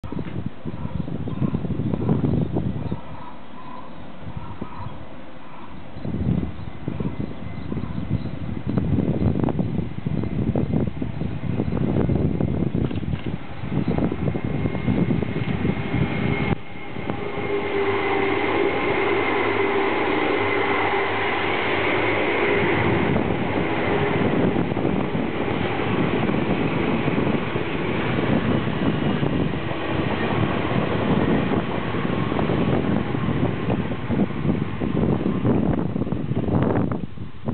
東急8500系幕車8606F つくし野通過
東急田園都市線8500系唯一の幕車、8606F回送のつきみ野駅通過のシーンです。